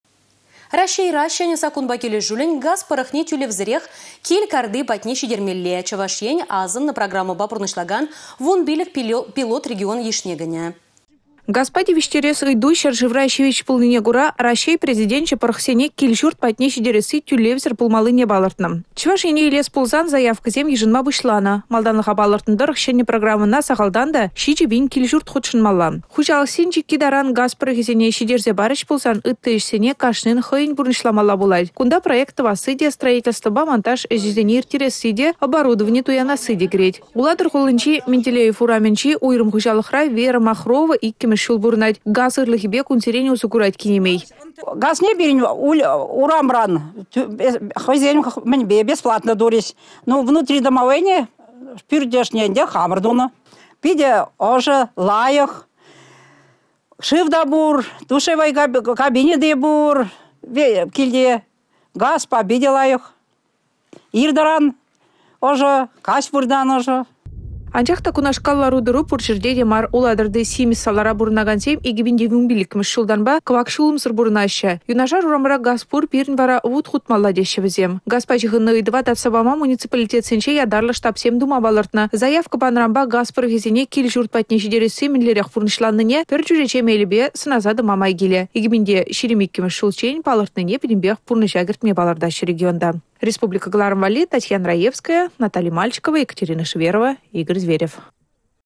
Газ кĕртмелли майсем пулмалла
Раççейре çĕнĕ саккунпа килĕшӳллен, газ пăрăхне тӳлевсĕрех кил карти патне çитермелле. Чăваш Ен асăннă программăна пурнăçланакан 15 пилот регон йышне кĕнĕ.